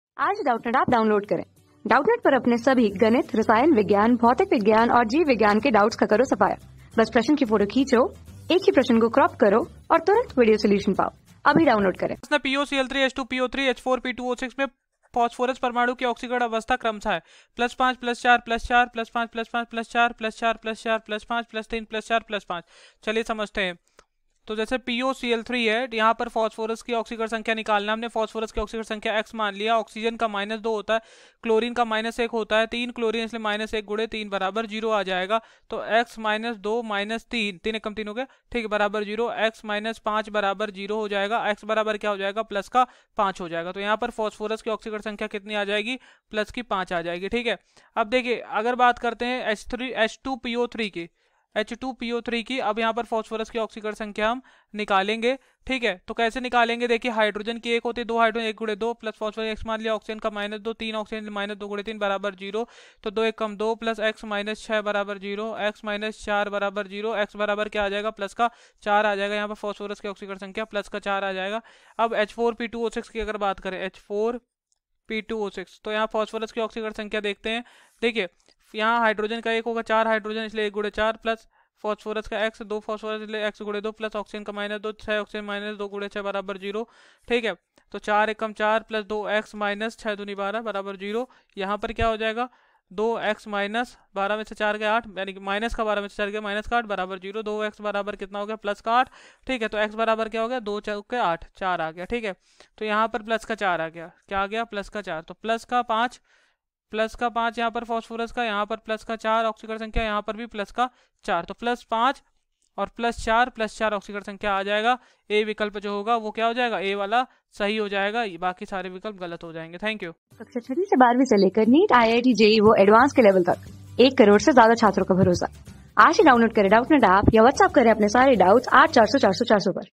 POCI_(3), H_(2)PO_(3) एवं H_(4)P_(2)O_(6) में P परमाणु की ऑक्सीकरण अवस्था क्रमश : है | 12
POCI_(3), H_(2)PO_(3) एवं H_(4)P_(2)O_(6) में P परमाणु की ऑक्सीकरण अवस्था क्रमश : है
Class: 12
Subject: CHEMISTRY
Chapter: p - ब्लॉक के तत्व (नाइट्रोजन, ऑक्सजीन, हैलोजन एवं उत्कृष्ट गैसें)
Board:NEET

👉 Have Any Query? Ask Us.